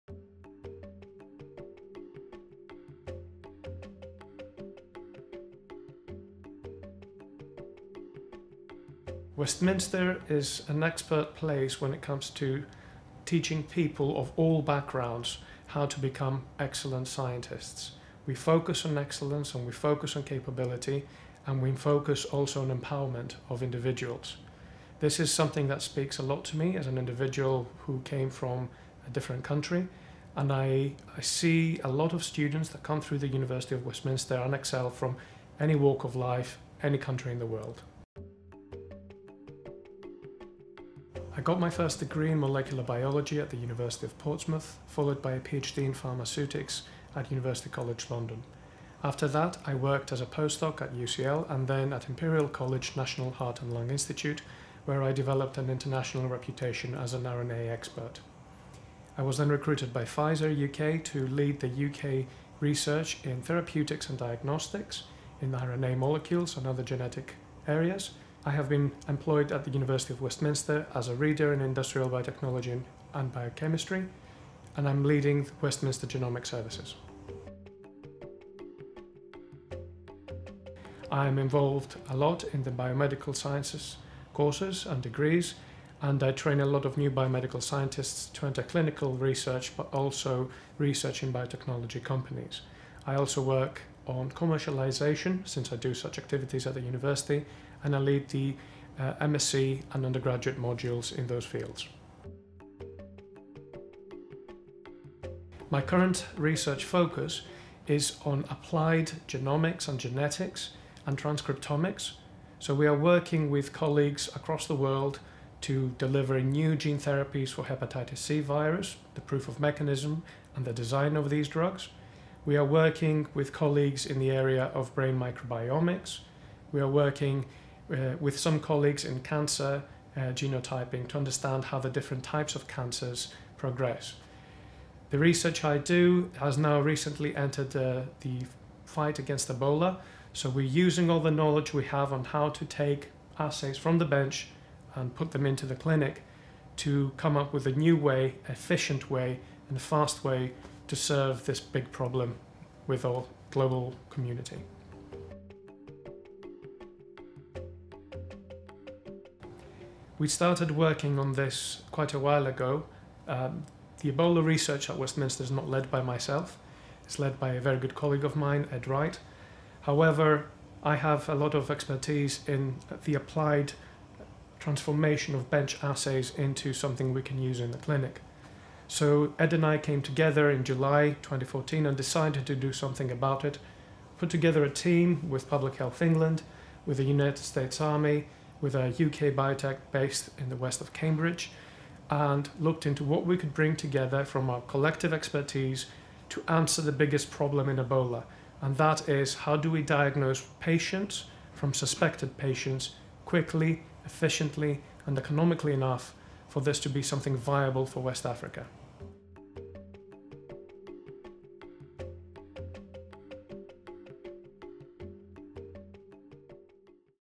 Meet Dr Sterghios A. Moschos from the Faculty of Science and Technology, University of Westminster
Dr Sterghios A. Moschos, Fellow of the Royal Society of Chemistry and Member of the Society of Biology, shares his view of what sets the University of Westminster apart from other universities, and talks about the Faculty of Science and Technology, his research interests in applied genetics, genomics and transcriptomics, and the University’s involvement in the fight against the Ebola virus.